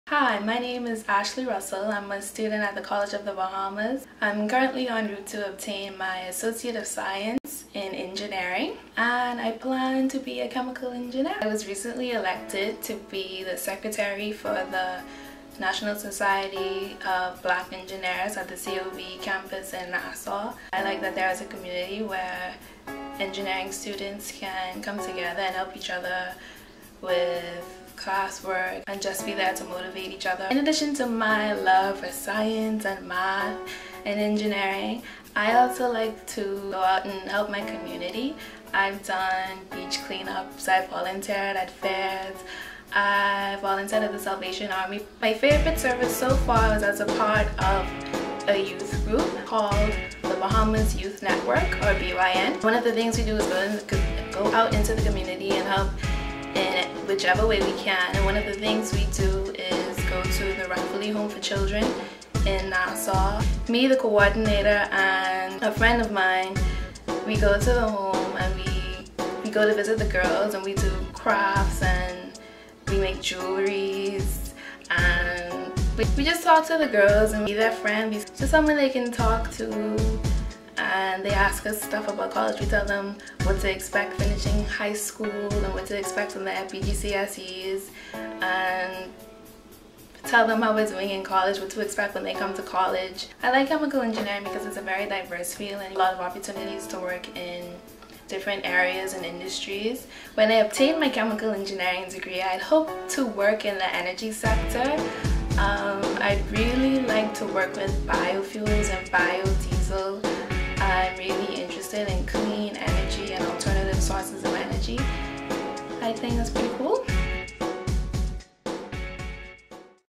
2014 Ziri Award Nominee - Ashley Russell
Ashley has always displayed a love for math and science. In high school, she was a member of the science club and took part in the SECME Mouse-Trap Car Competition and the Battle of the Brains competition in Grand Bahama. In tenth grade, knowing she wanted to pursue a career in science, Ashley opted to take chemistry and physics classes. She graduated high school passing her Math, Physics, Biology, Chemistry and Combined Science BGCSE's with A's. After thinking long and hard, Ashley decided that she wanted to be a chemical engineer.
Ashley enrolled in the Engineering programme at The College of The Bahamas in the Fall of 2012. She started her studies at the Freeport campus where she was a member of the Math Club and is currently continuing her studies at the Nassau campus where she is a member of the National Society of Black Engineers of which she was recently elected Secretary for the next year. Ashley hopes to eventually work as a chemical engineer in biofuels and bioenergy, to promote clean energy.
Ashley loves to give back to her community. She is currently a member of the Bahamas Youth Network youth group. As a part of the group, Ashley visits the Ranfurly Home for Children two to four times a month where she takes part in craft activities with the girls of the Ranfurly Home, while offering various forms of advice and just being someone the girls can talk to.

"Ashley lends her ear to anyone who needs it and offers advice to those who request it."